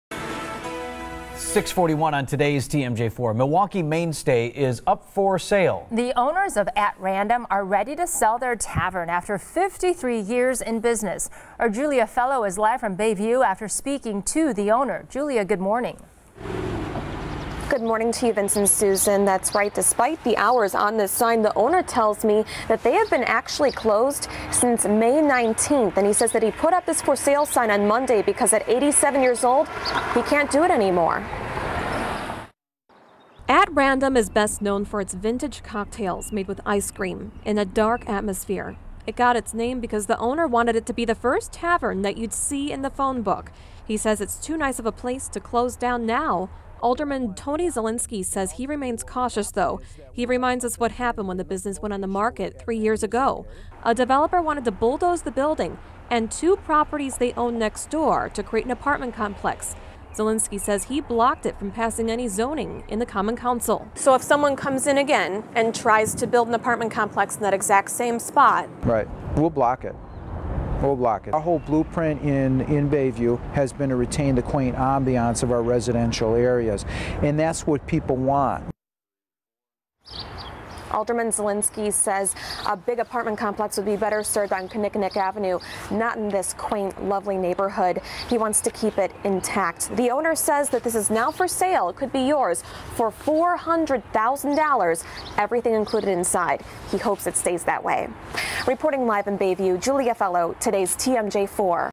"At Random" in Milwaukee's Bay View neighborhood could be yours for $400,000
The owners of "At Random" in Milwaukee's Bay View neighborhood are ready to sell their tavern, after more than 50 years in business.